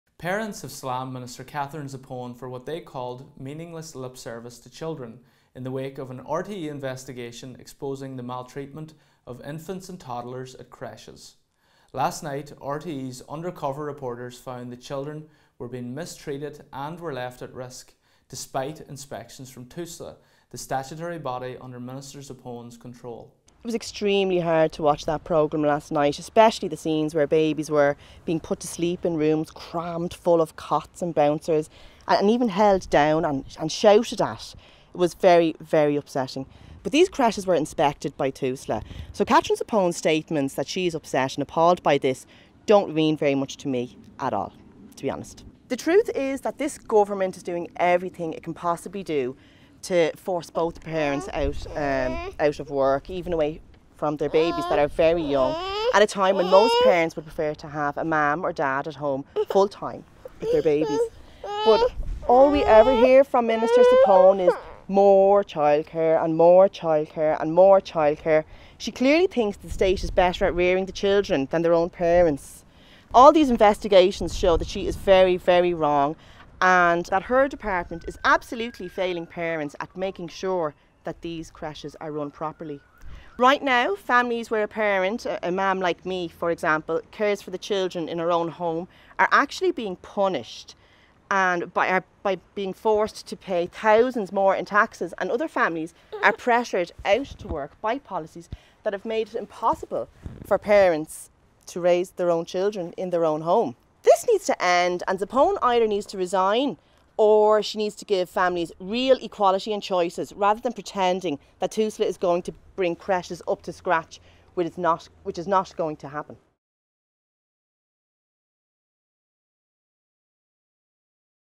Parents hit back at Minister Zappone: Comments on RTE investigates creche exposure are meaningless
Parents have hit back at Minister Katherine Zappone, saying her comments on the RTE investigates creche exposure are meaningless as long as the government policies remain fixated on childcare while punishing parents who raise their own children at home.